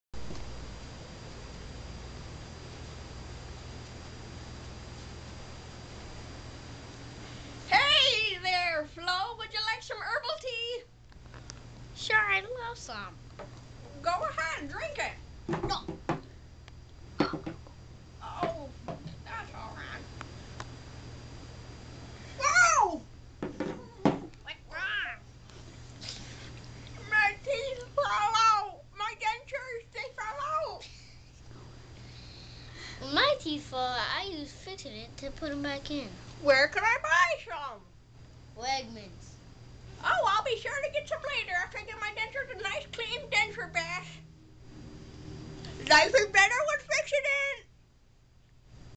OLD LADY DENTURE COMMERCIAL
RingoPingo4 & Nancyandgertrude98 do a commercial about dentures.